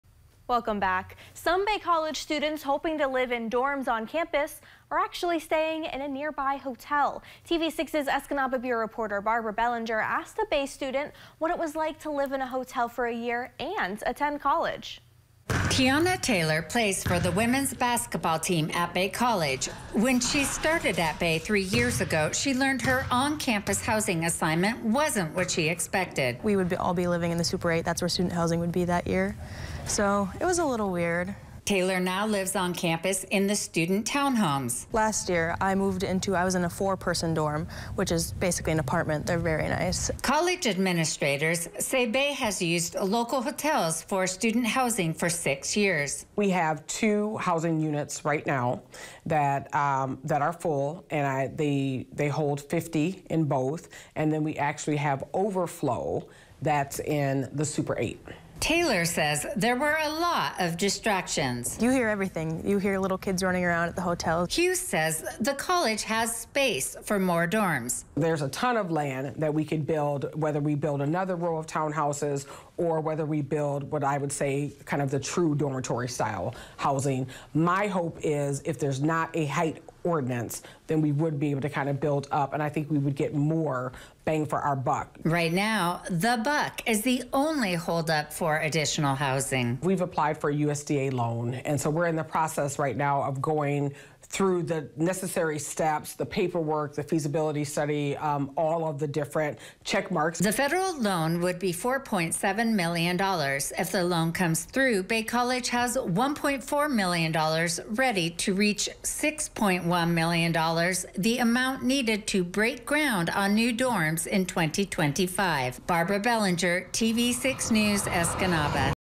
Bay College uses hotel for overflow student housing
Some Bay College students hoping to live in dorms on the Escanaba campus are actually staying in a nearby hotel.